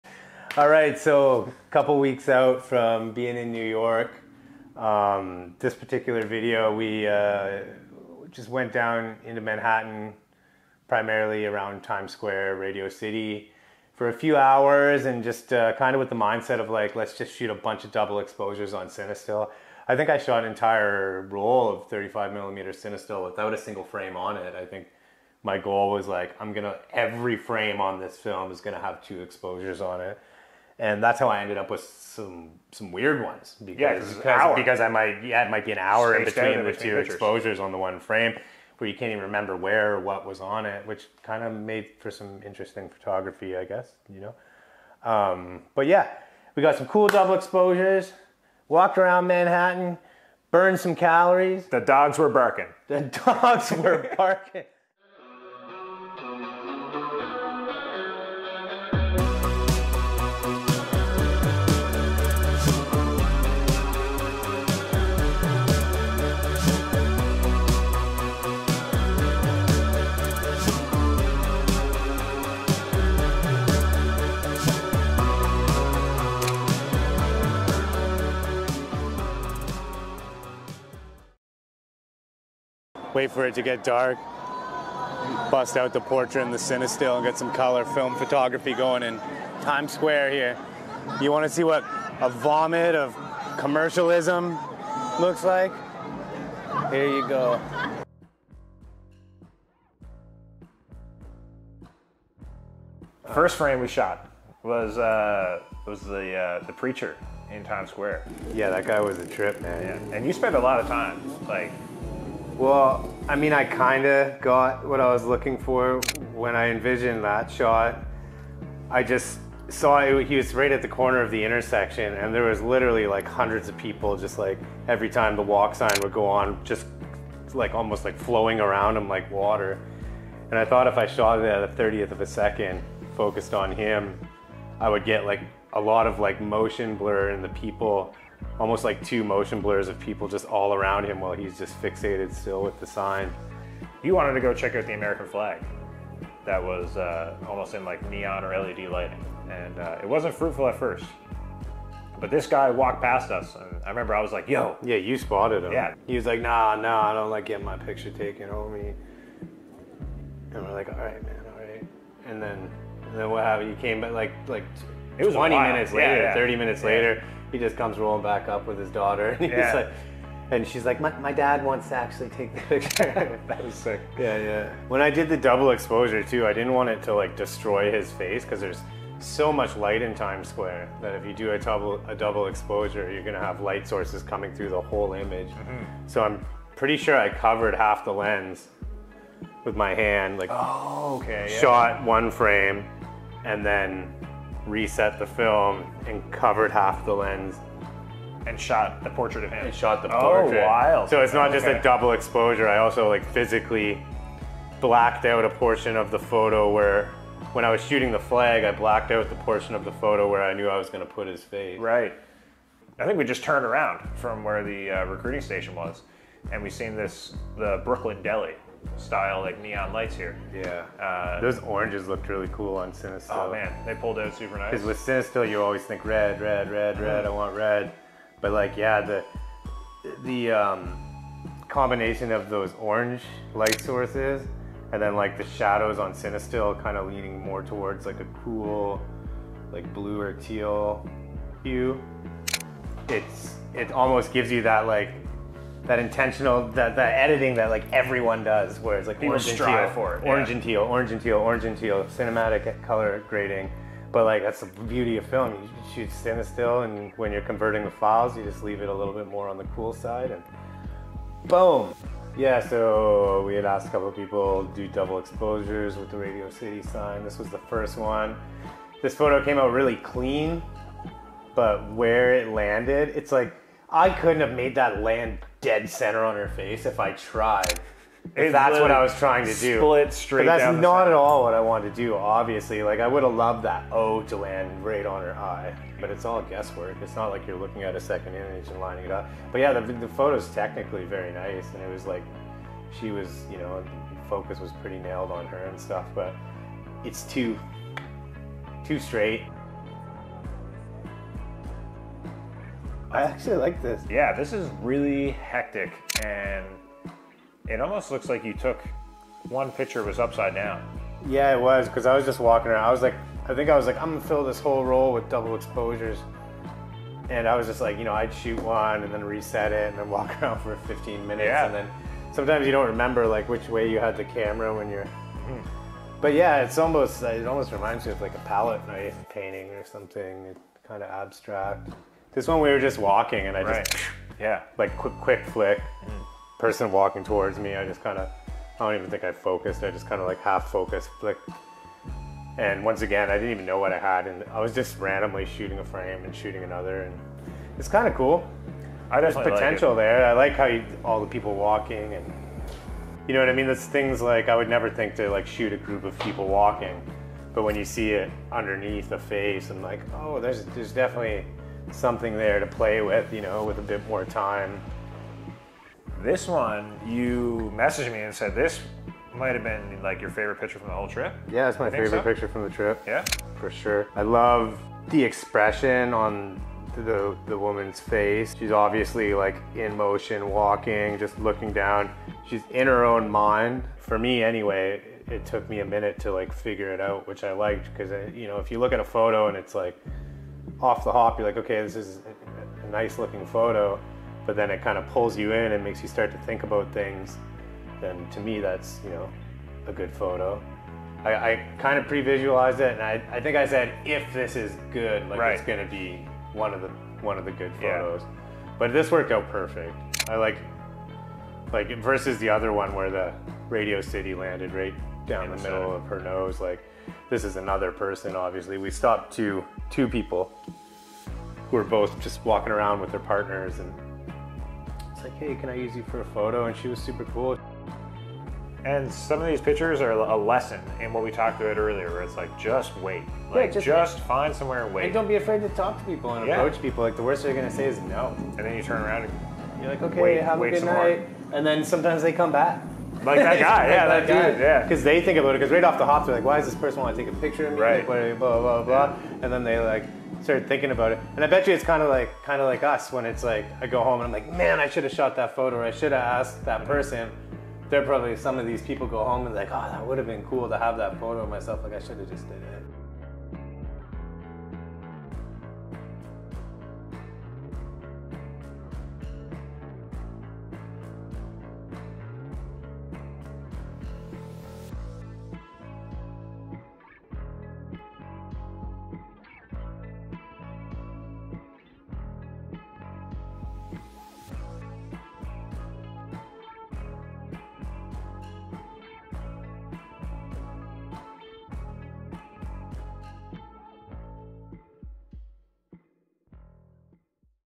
EP. 6 Time Square on CINESTILL 800t // Olympus OM-1 & Rolleiflex 3.5f Double Exposures in NYC
During our trip to NYC, we headed to time square to take some double exposures and spend a few rolls of CINESTILL 800t.
Shot on the Olympus OM-1 and Rolleiflex 3.5f